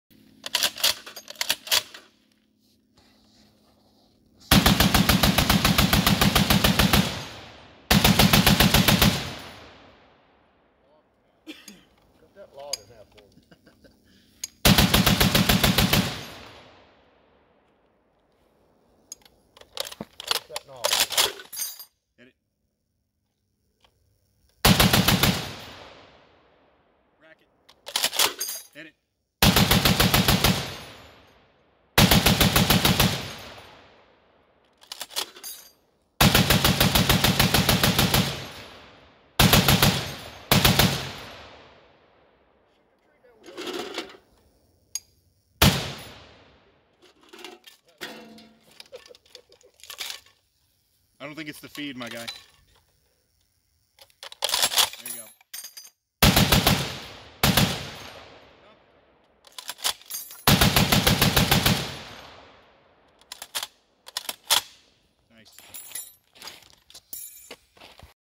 Ma Deuce M2HB .50 Cal BMG Belt-Fed Full Auto
Had some light primer strikes in this can